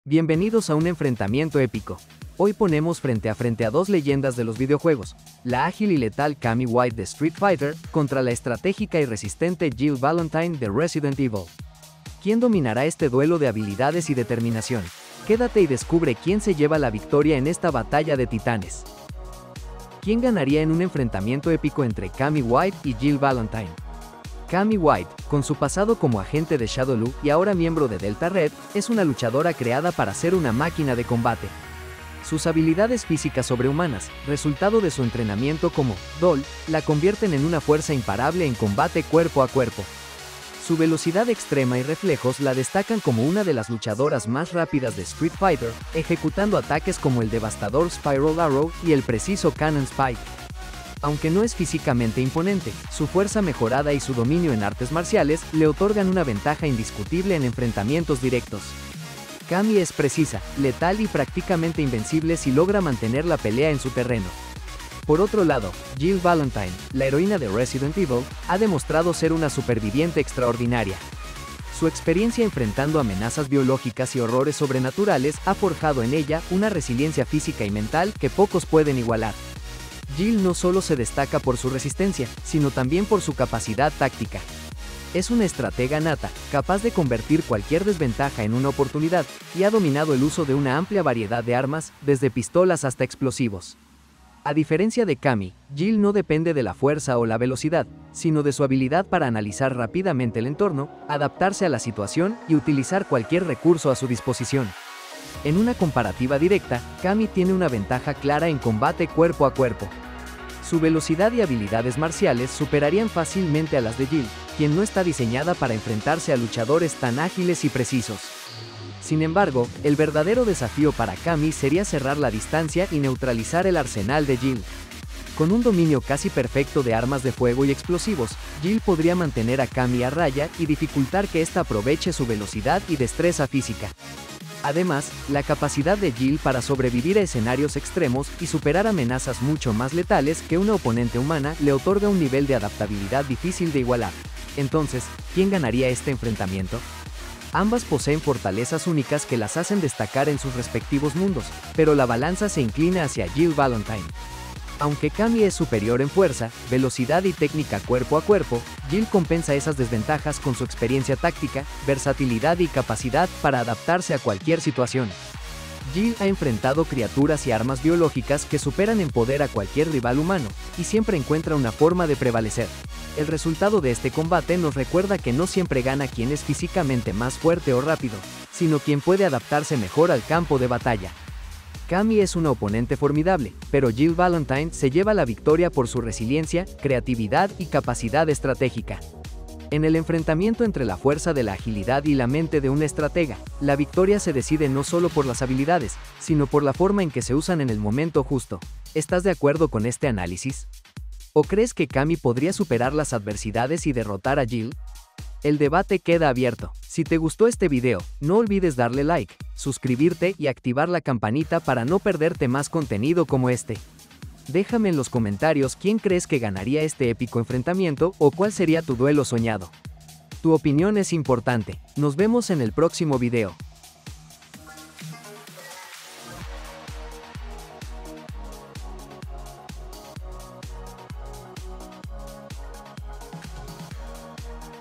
Cammy vs Jill: ¡El Enfrentamiento Épico que Nunca Imaginaste! 🤯
En este video analizamos un épico enfrentamiento entre dos íconos de los videojuegos: Cammy White de Street Fighter y Jill Valentine de Resident Evil. Exploramos sus historias, habilidades y lo que las convierte en rivales únicas. ¿Quién tiene lo necesario para llevarse la victoria? Descúbrelo y únete a la discusión en los comentarios.